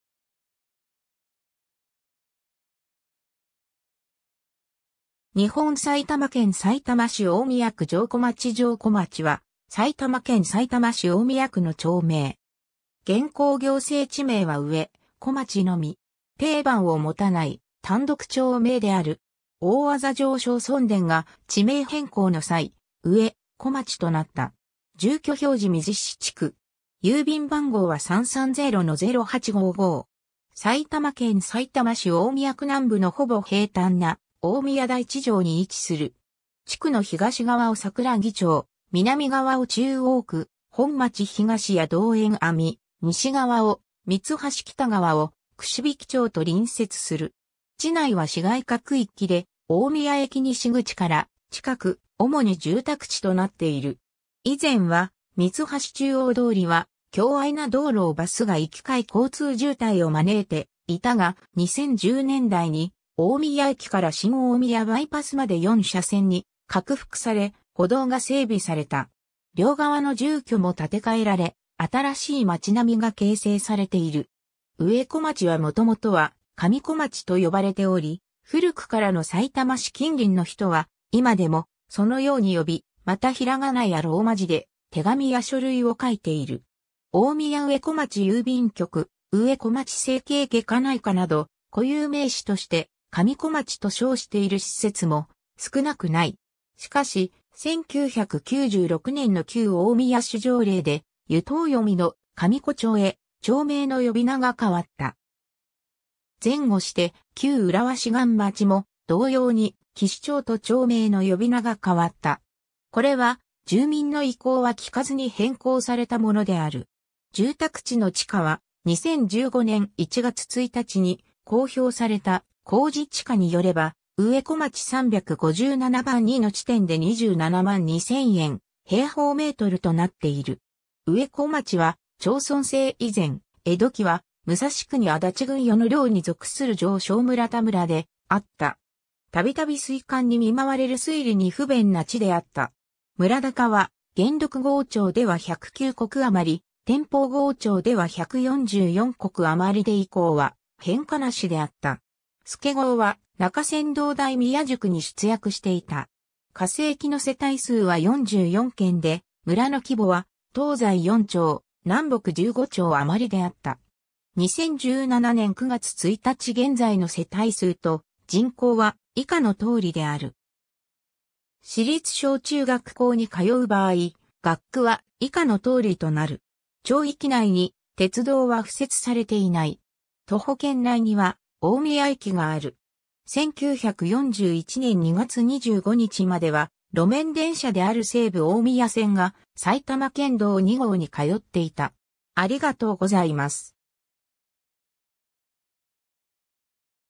上小町
大宮区の町・字
日本 」 埼玉県 」 さいたま市 」 大宮区 」 上小町 上小町（かみこちょう）は、埼玉県さいたま市大宮区の町名。
現行行政地名は上小町のみ。
大字上小村田が地名変更の際、上小町となった。
住居表示未実施地区。
 埼玉県さいたま市大宮区南部のほぼ平坦な大宮台地上に位置する。
地区の東側を桜木町、南側を中央区本町東や同円阿弥、西側を三橋北側を櫛引町と隣接する。
地内は市街化区域（主に第一種住居地域）で大宮駅西口から近く、主に住宅地となっている。
以前は三橋中央通りは狭隘な道路をバスが行き交い交通渋滞を招いていたが、2010年代に大宮駅から新大宮バイパスまで4車線に拡幅され、歩道が整備された。
両側の住居も建て替えられ、新しい街並みが形成されている。
 上小町はもともとは「かみこまち」と呼ばれており、古くからのさいたま市（大宮市や浦和市）近隣の人は、今でもそのように呼び、また平仮名やローマ字で手紙や書類を書いている。
大宮上小町郵便局（かみこまちゆうびんきょく）、上小町整形外科内科（かみこまちせいけいげかないか）など、固有名詞として「かみこまち」と称している施設も少なくない。
 しかし、1996年（平成8年）の旧大宮市条例で湯桶読みの「かみこちょう」へ町名の呼び名が変わった。
前後して、旧浦和市岸町（きしまち）も同様に「きしちょう」と町名の呼び名が変わった。
これは住民の意向は聞かずに変更されたものである（上小町自治会だより第110号、第111号参照。）。
 住宅地の地価は、2015年（平成27年）1月1日に公表された公示地価によれば、上小町357番2の地点で27万2000円/m2となっている。
 上小町は町村制以前、江戸期は武蔵国足立郡与野領に属する上小村田村であった。
たびたび水旱に見舞われる水利に不便な地であった。
村高は『元禄郷帳』では109石余、『天保郷帳』では144石余で以降は変化なしであった。
助郷は中山道大宮宿に出役していた。
化政期の世帯数は44軒で、村の規模は東西4町、南北15町余であった。
 2017年（平成29年）9月1日現在の世帯数と人口は以下の通りである。
 町域内に鉄道は敷設されていない。
徒歩圏内には大宮駅がある。
1941年（昭和16年）2月25日までは路面電車である西武大宮線が埼玉県道2号に通っていた（旧町域内への駅の設置はなし）。